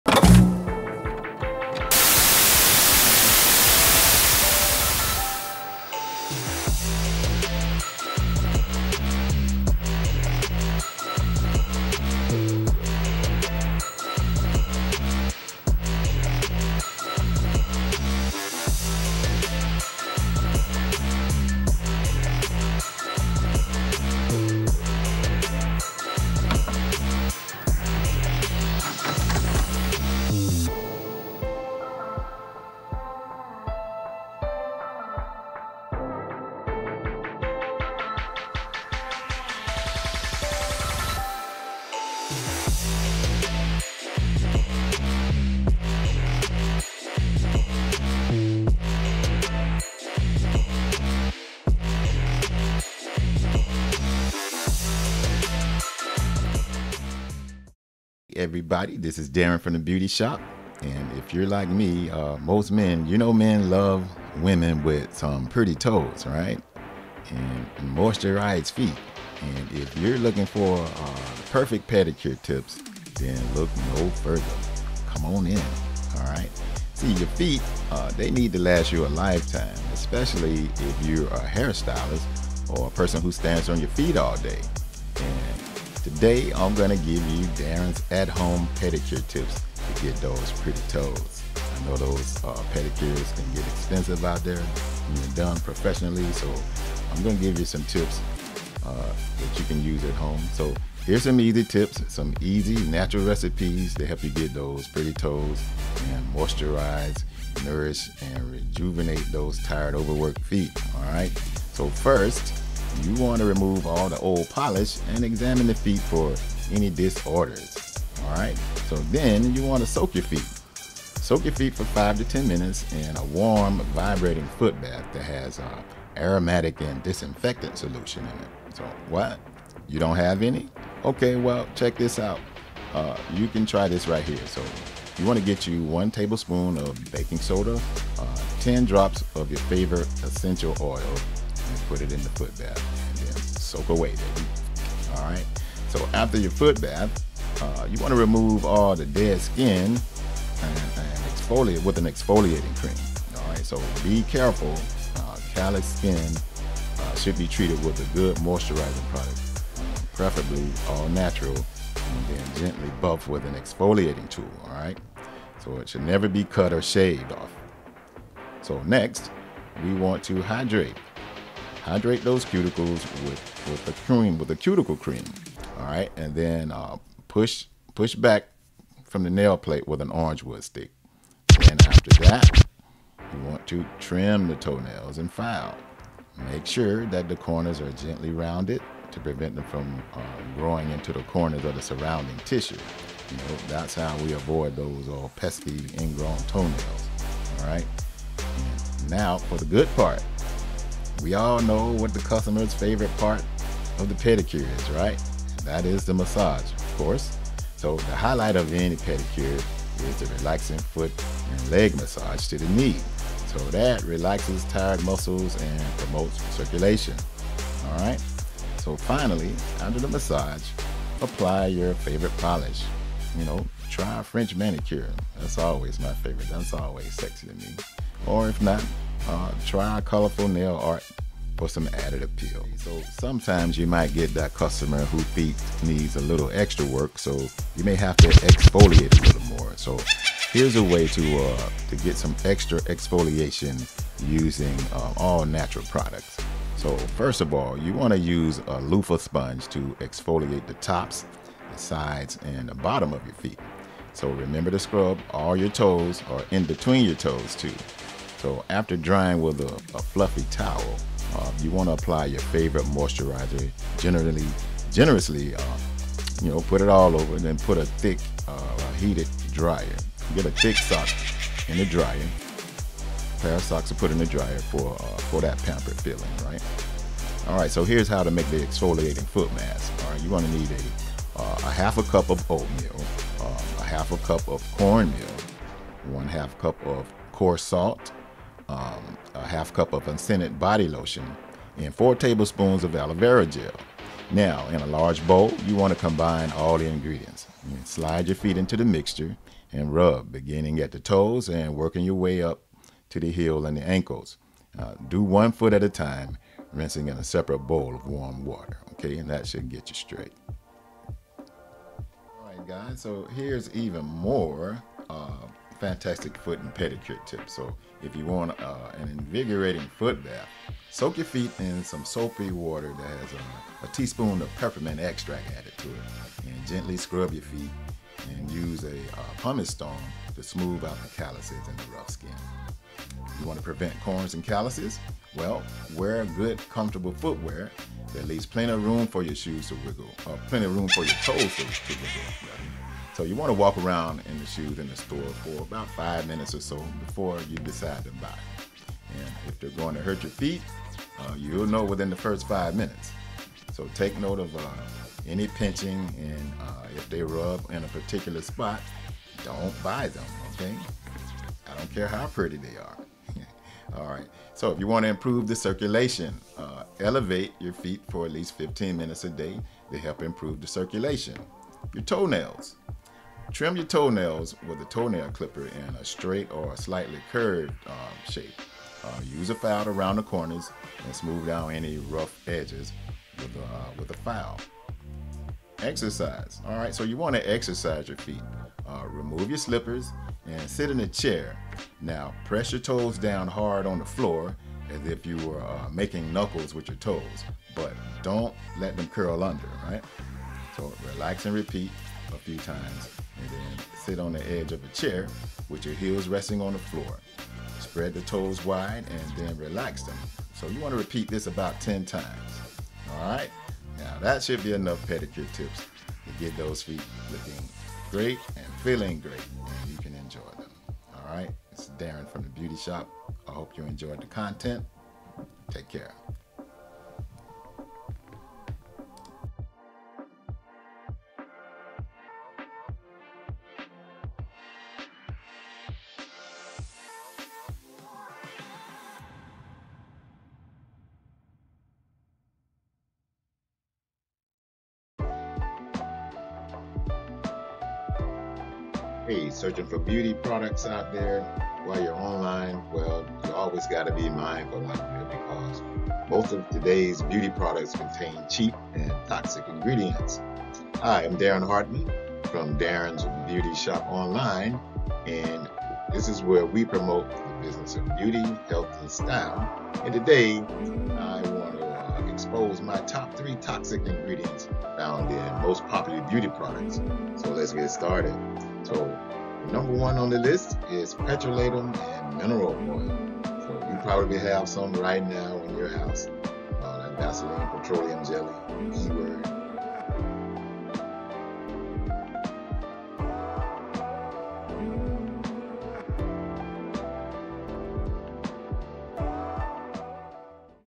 Perfect Pedicure Tips You Should Know
Welcome to Darron's Beauty Shop Online. In today's video, we're diving into the art of achieving the perfect pedicure using natural products right in the comfort of your own home. Say goodbye to expensive salon visits and hello to an indulgent spa experience that your feet truly deserve!
In this video, we'll guide you through the secrets of a flawless pedicure, all while embracing the goodness of nature. From relaxing foot soaks to revitalizing scrubs and nourishing masks, we've curated a routine that will leave your feet looking and feeling rejuvenated.